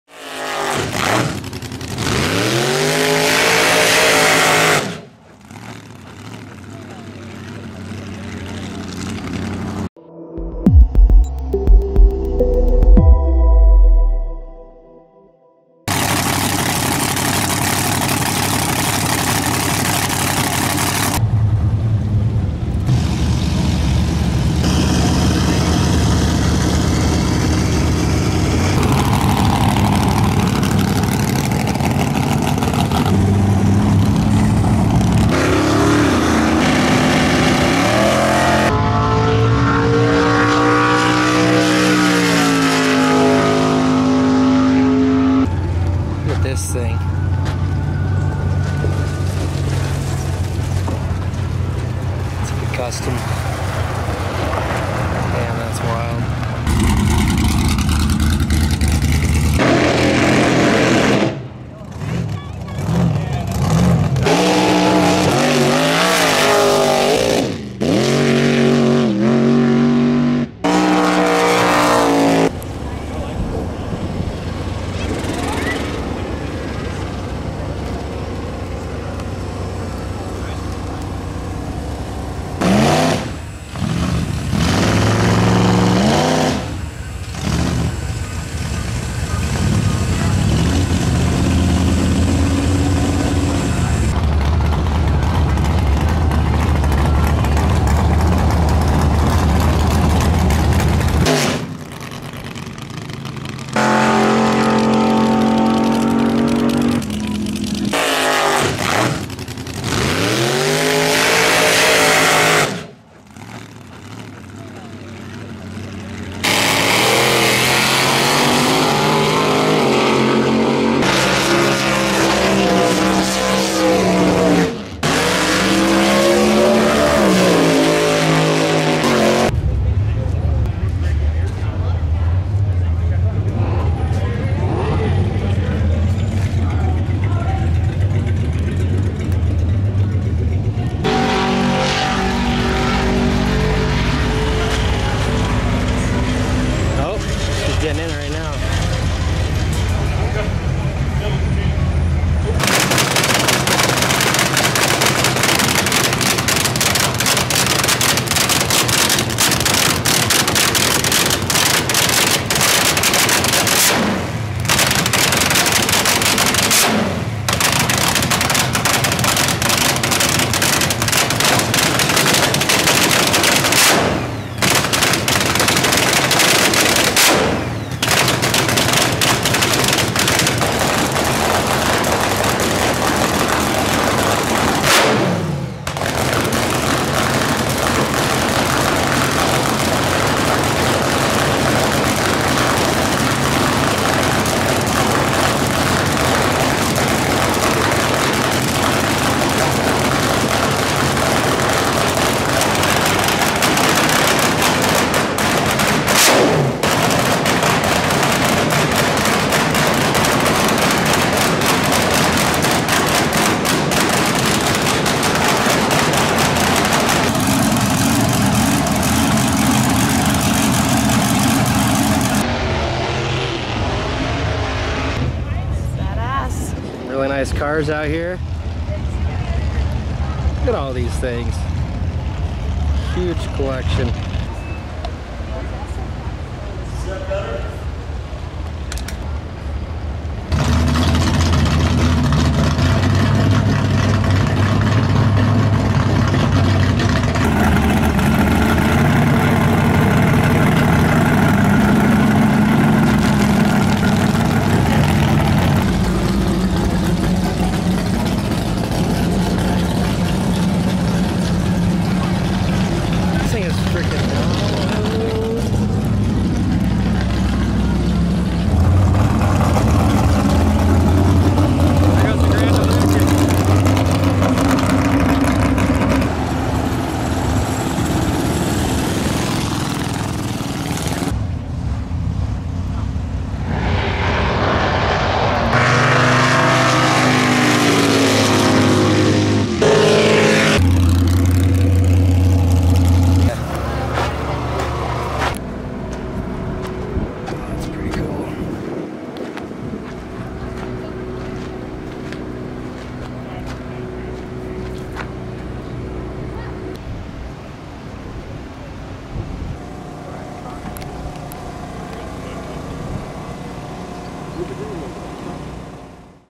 2019 Tri-State Hot Rod Revival | Nostalgia Drag Racing
Walking around the pits at the 2019 Tri-State Hot Rod Revival. The event gets bigger each year, and it's incredible to see so many legendary drag cars being shown off and raced down the strip! Getting to sit in the Nitro front engine dragster was definitely the highlight! Can't wait for next year!